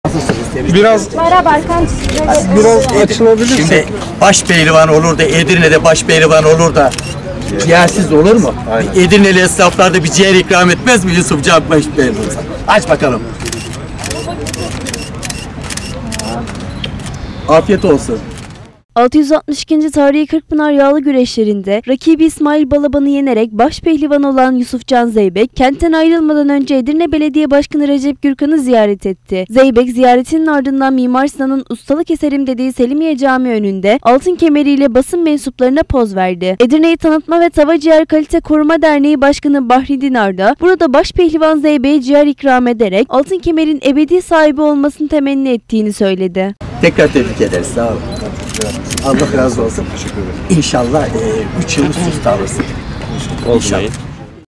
DİNAR’DAN BAŞPEHLİVANA CİĞER İKRAMI
Edirne’yi Tanıtma ve Tava Ciğer Kalite Koruma Derneği Başkanı Bahri Dinar, 662’nci Tarihi Kırkpınar Yağlı Güreşleri’nin Başpehlivanı olan Yusuf Can Zeybek’e Selimiye Camii önünde ciğer ikram etti.